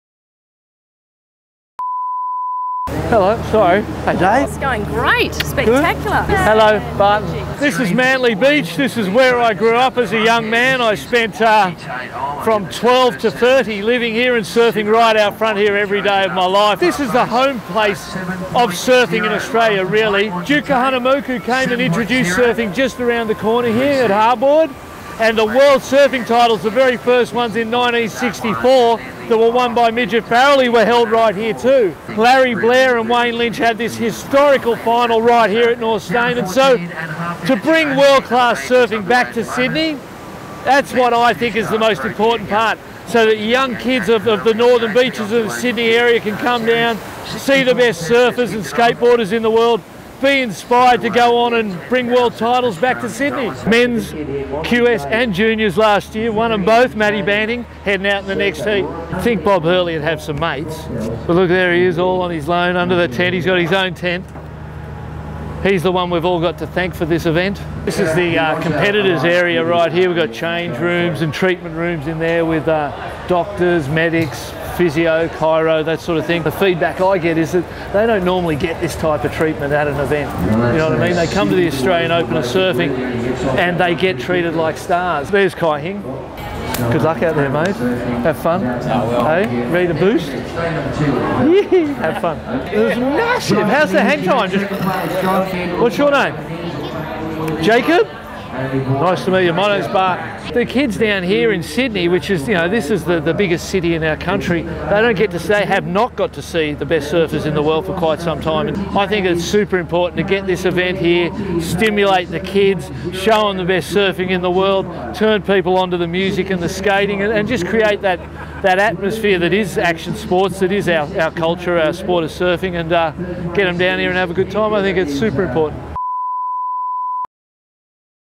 TOURNOTES: AUSTRALIAN OPEN OF SURFING WITH BARTON LYNCH
Contest director Barton Lynch takes behind the scenes at the Hurley Australian Open of Surfing Presented by Nike SB